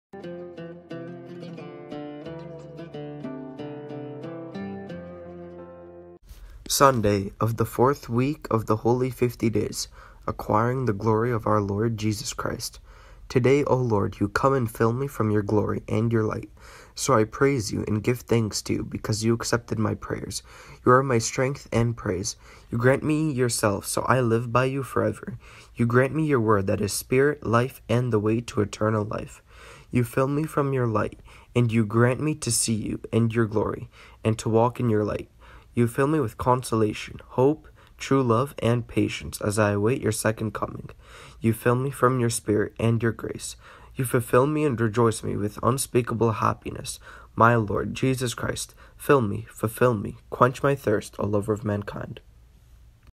Monastic contemplation according to the daily readings, Sunday of the Fourth week of the Holy Fifty
Sunday of the Fourth week of the Holy Fifty Days (Acquiring the glory of our Lord Jesus Christ)
Today, O Lord You come and fill me from Your glory and Your light,
So I praise You and give thanks to You because You accepted my prayers, You are my strength and praise.
You grant me Yourself so I live by You forever,
You grant me Your word that is spirit, life, and the way to eternal life,
You fill me from Your light and You grant me to see You and Your glory, and to walk in your light.
You fill me with consolation, hope, true love and patience as I await Your second coming,
You fill me from Your Spirit and Your grace,
You fulfill me and rejoice me with unspeakable happiness,
My lord Jesus Christ… fill me. ..Fulfill me...Quench my thirst O Lover of mankind